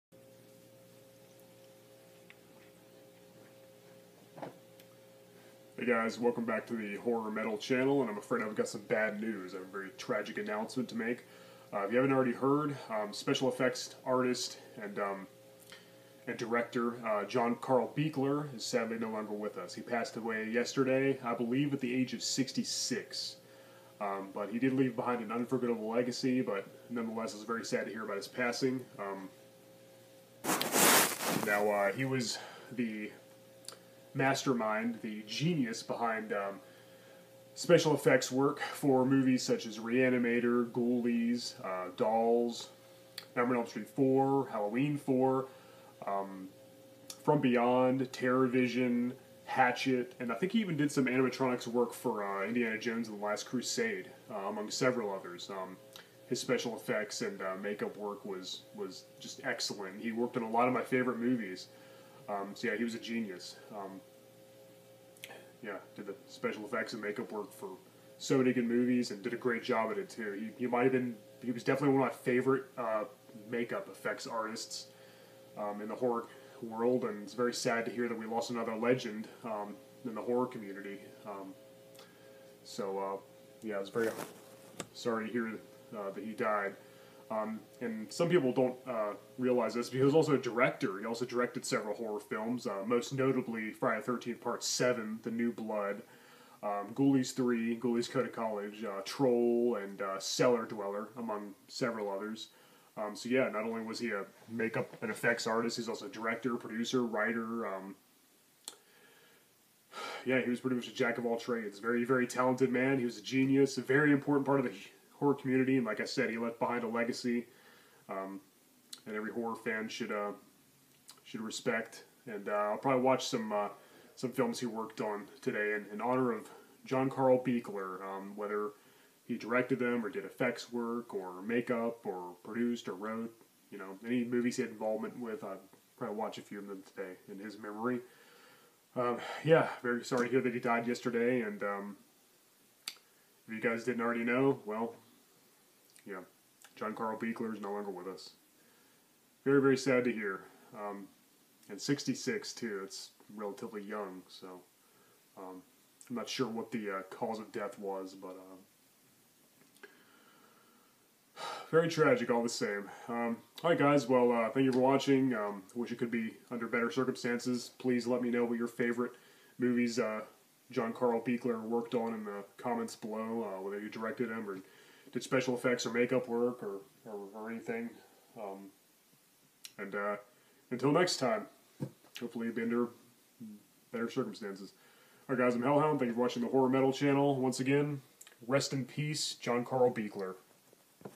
R.I.P. John Carl Buechler
We lost another legend in the horror community today, special effects/makeup artist and director John Carl Buechler is sadly no longer with us. This is my tribute to him.